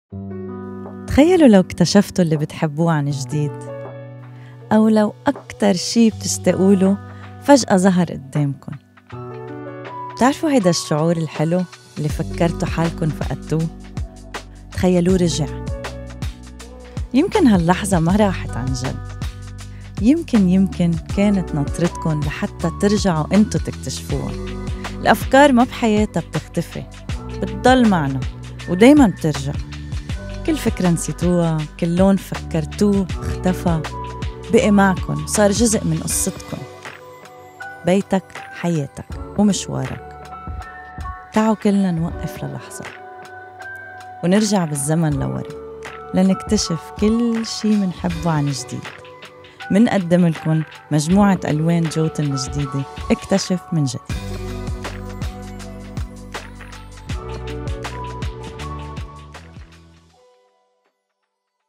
جوتن | مجموعة الألوان 2021 – اكتشف من جديد
اكتشف كل لحظة في خيالك من جديد، بألوان عادت من الماضي لتلتقي بالحاضر. تعرّف على مجموعة موضة الألوان لدينا.